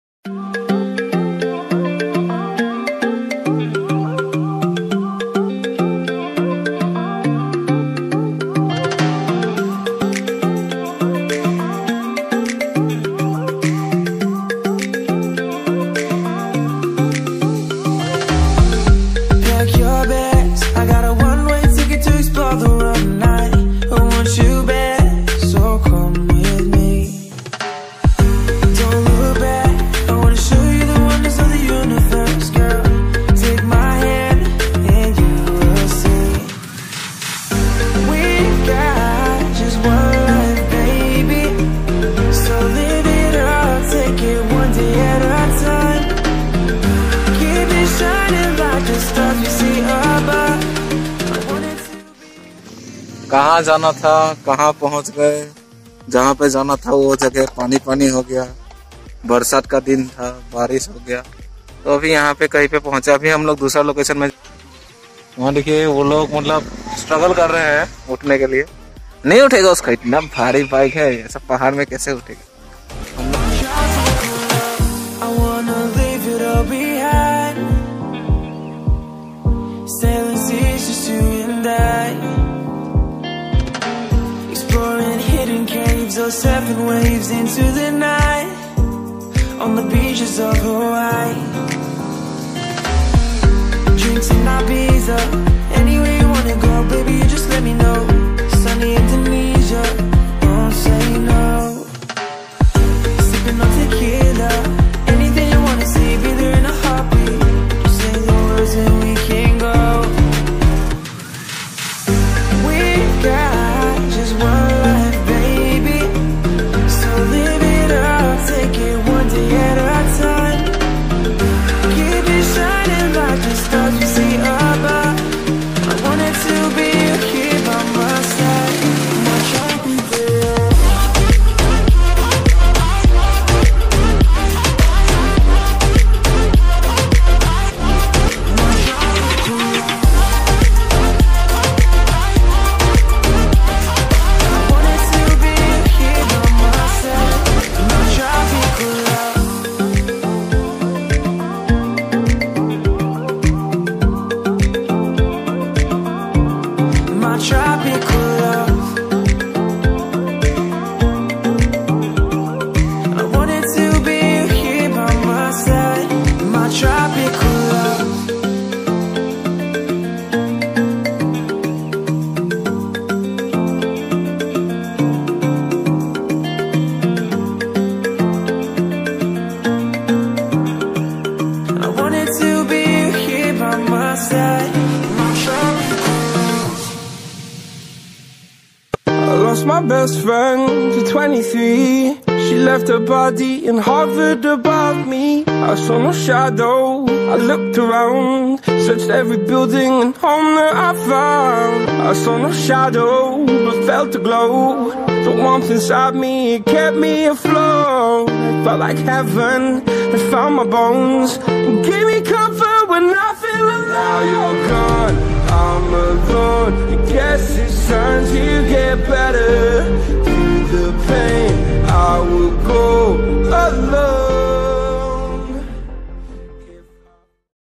Sunday Outing with friends
At first we were planning to go to Govali Gaon, but Due to heavy rainfall the location we were about to go got Floded , hope you guys enjoy it